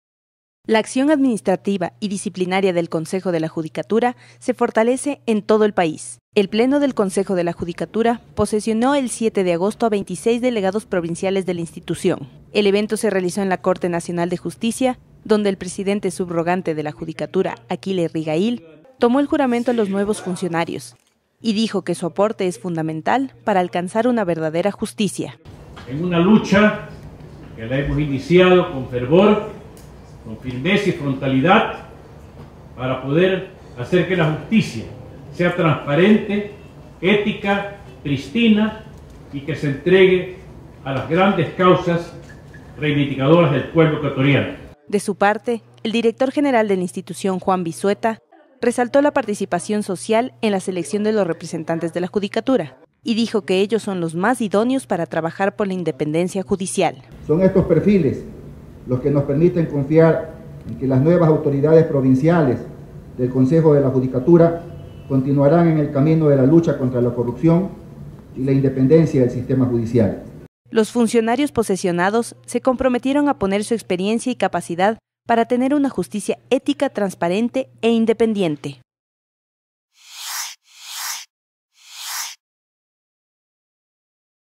DELEGADOS PROVINCIALES DEL CONSEJO DE LA JUDICATURA ASUMIERON SUS FUNCIONES
El Pleno del Consejo de la Judicatura posesionó, el 7 de agosto, a 26 delegados provinciales de la institución.
Ellos fueron designados luego de un análisis riguroso de su trayectoria y de un proceso de impugnación ciudadana.
Universidades y organizaciones sociales de todo el país participaron en la selección de los representantes de la Judicatura.